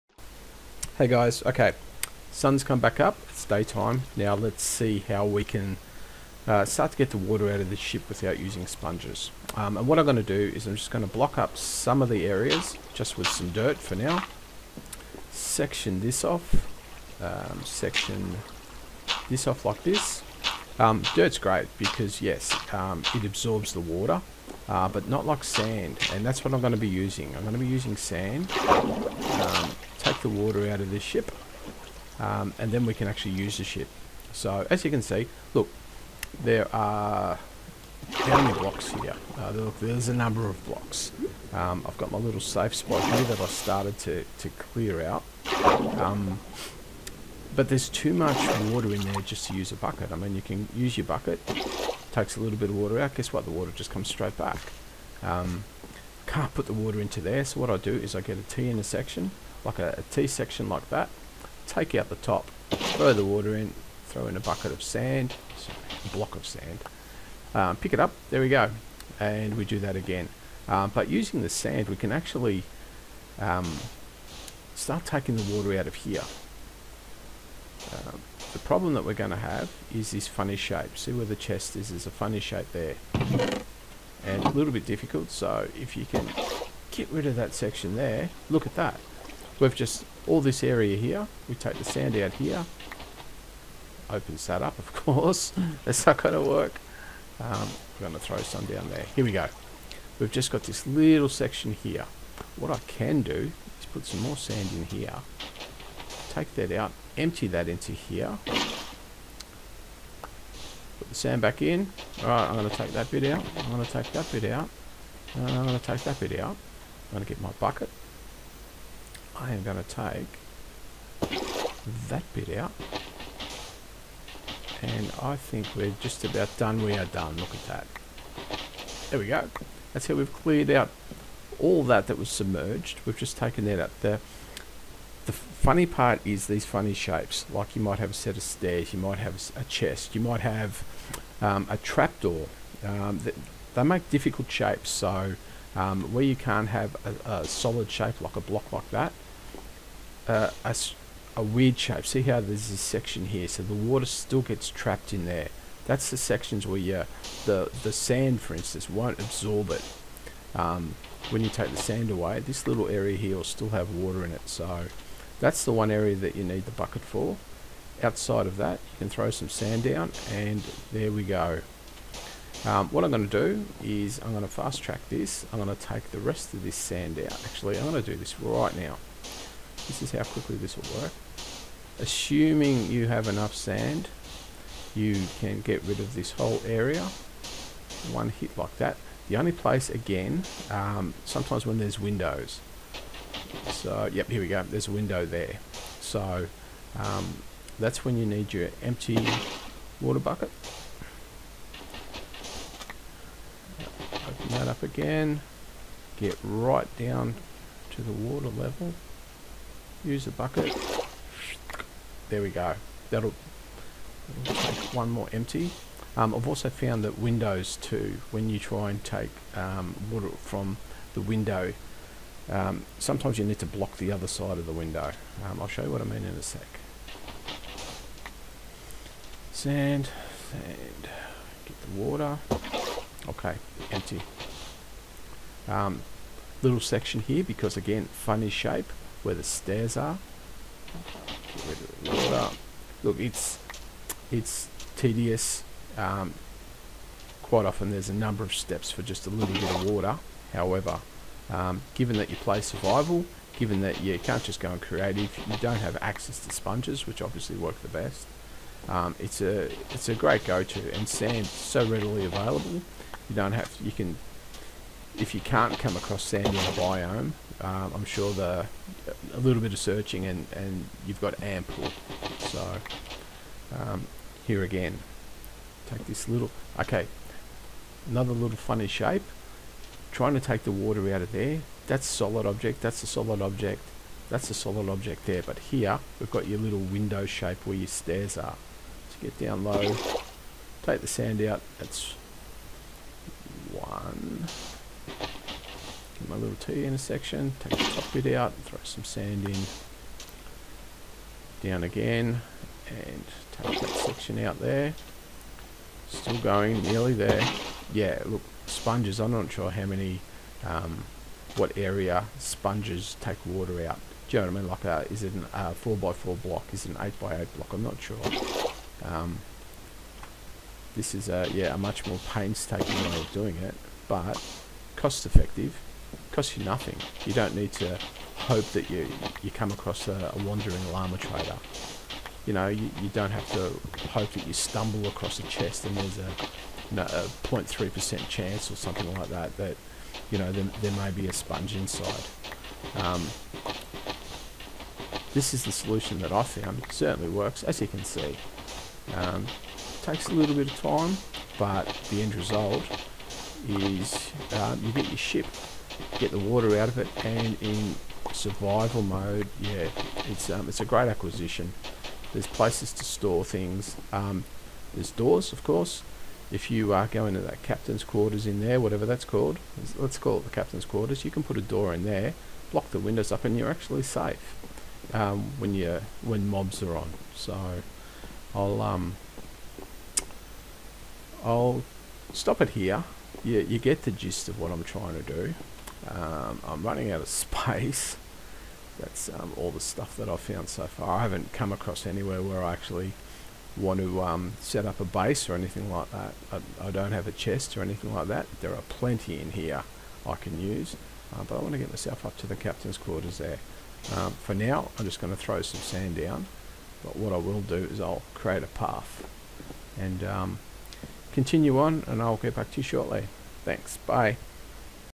Removing water from Sunken Ship - Survival - pt 2
The leaks in the Sunken Ship have been filled. It's time to start removing the water. Watch as I remove the water from within the ship without the need for sponges - all in Survival mode with resources that are readily available.